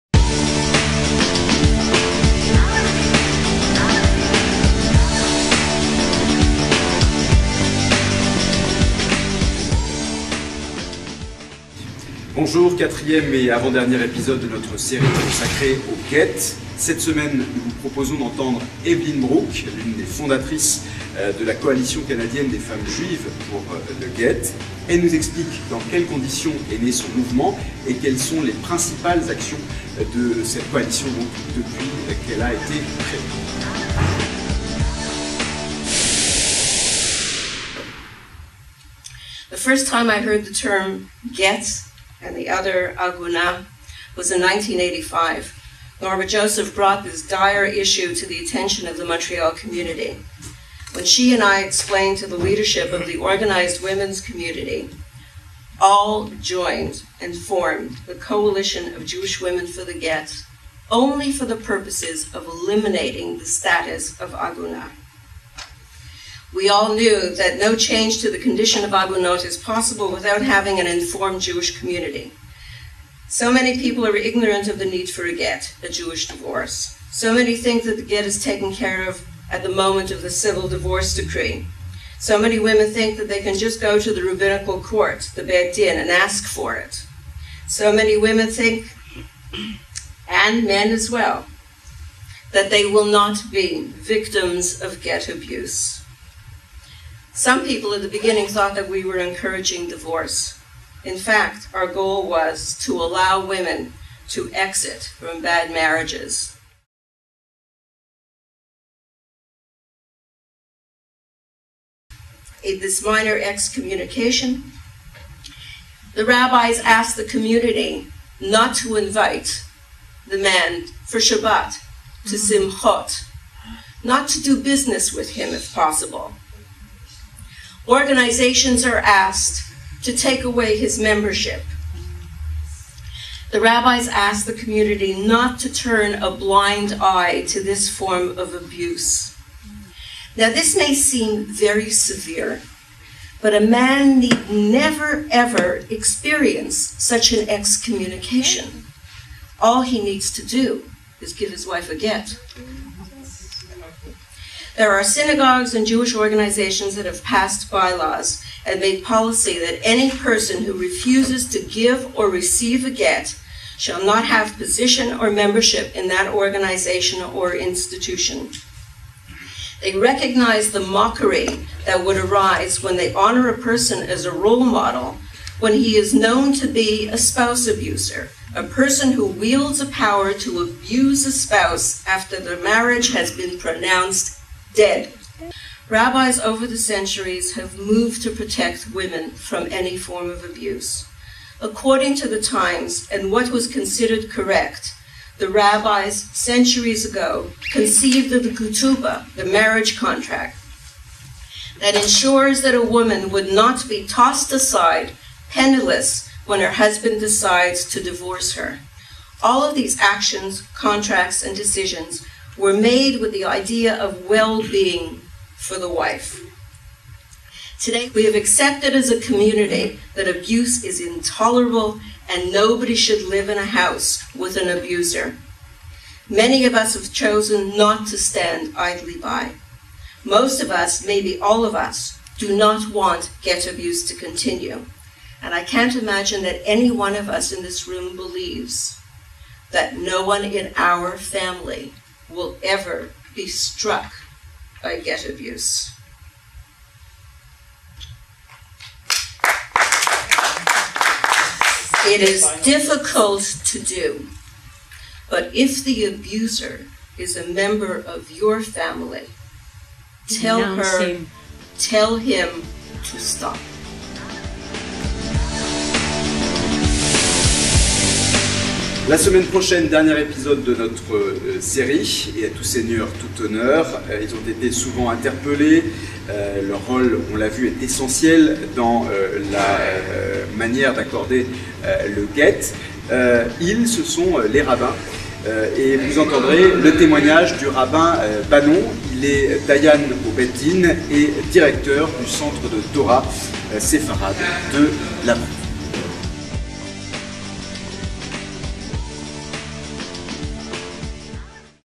Le divorce religieux partie 4
Du 28 juillet au 29 août, la CSUQ vous propose une série vidéo exceptionnelle via LVS.tv, sur son site web, à propos du divorce religieux. Dans cette première partie, vous allez entendre Diane Sassoun, Présidente d'Auberge Shalom, qui évoque un cas douloureux de femme en attente du Guet. Depuis presque vingt ans, l'Auberge Shalom accueille ces femmes battues par leurs maris ou leurs conjoints. Et chaque année, ce sont une centaine de ces femmes que l'association a hébergé et aidé à se reconstruire. Une association dirigée par Diane Sassoun. "Le divorce religieux en question" est un feuilleton en cinq épisodes fait intervenir les témoingnages des intervenantes et intervenants présents à la table ronde organisée sur le sujet, le 16 juin dernier, lors du festival Sefarad 2008. Cette série pose le problème des femmes « agounot », en attente de leur « guet » ; problème qui figure comme l'un des plus cruciaux que connaît le judaïsme actuel. Les témoignages évoqueront les possibilités d'évolution de la loi juive à ce sujet ainsi que l'usage qui peut être fait des lois civiles, dans le monde et plus particulièrement au Canada pour aider les femmes « agounot ». Partie 3 : Maître Annie Dreyfus, spécialiste du droit de la famille et fondatrice du centre de formation du droit des femmes, évoque les aspects juriques pour l'obtention du guet